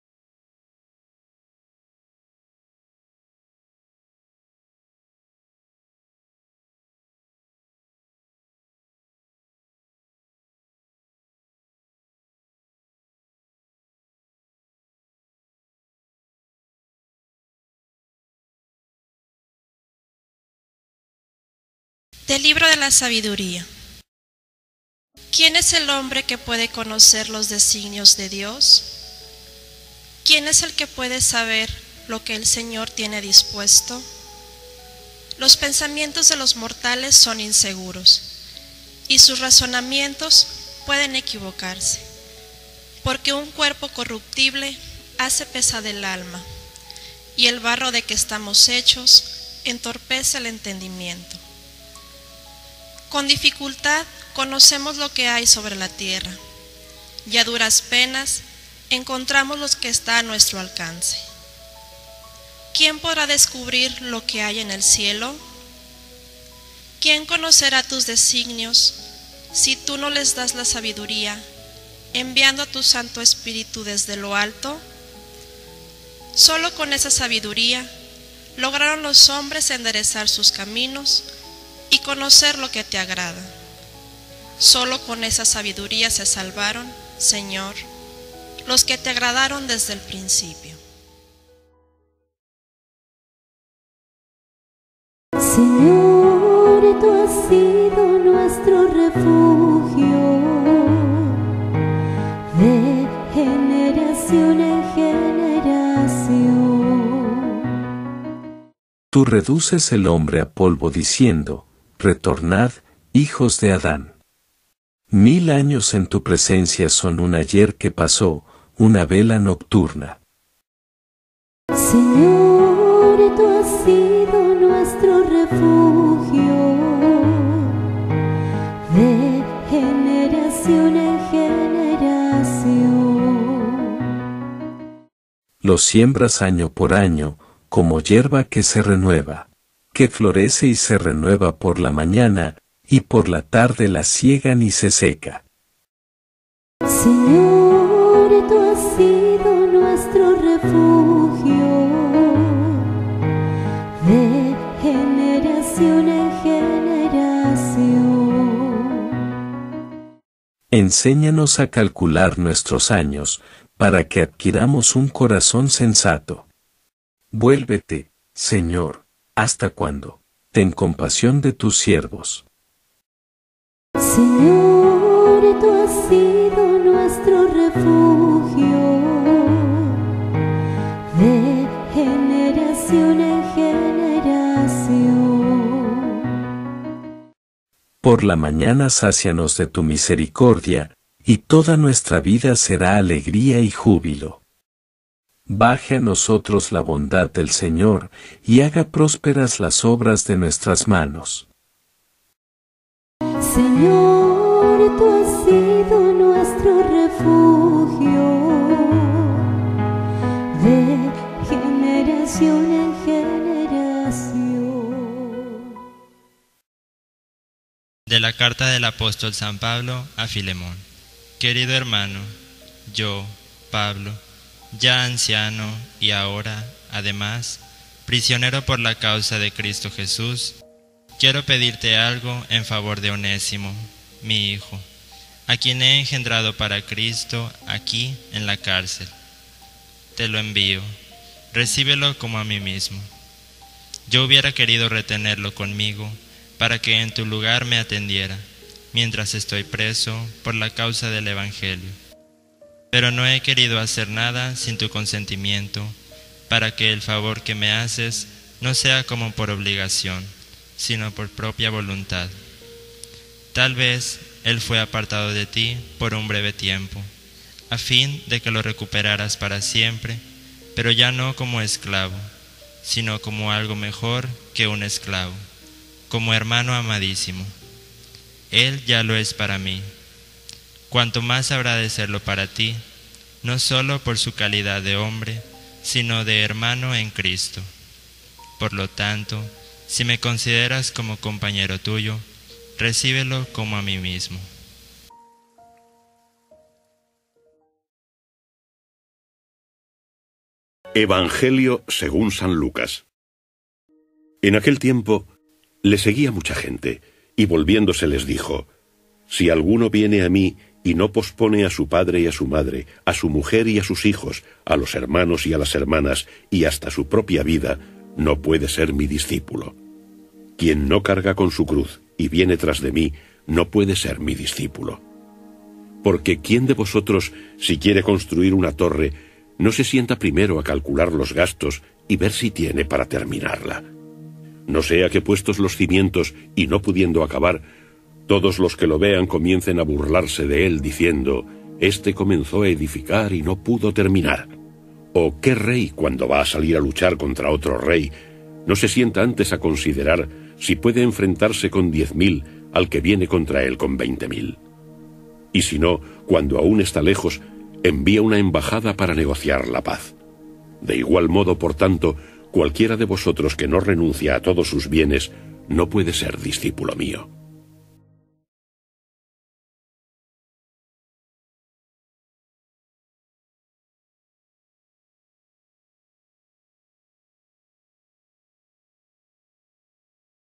Evangelio del día Domingo 4 de Septiembre de 2022| LUCAS 14,25-33
Evangelio y Lecturas santa misa Domingo Septiembre 4 DE 2022 DOMINGO XXIII VIGÉSIMA TERCERA SEMANA DEL TIEMPO ORDINARIO PRIMERA LECTURA: Del libro de la Sabiduría 9,13-18 SALMO 89, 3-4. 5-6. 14 y 17 R/.Señor, tú has sido nuestro refugio de generación en generación SEGUNDA LECTURA De la carta del apóstol san Pablo a Filemón 9b-10.12-17 EVANGELIO Del Santo Evangelio según San Lucas 14, 25-33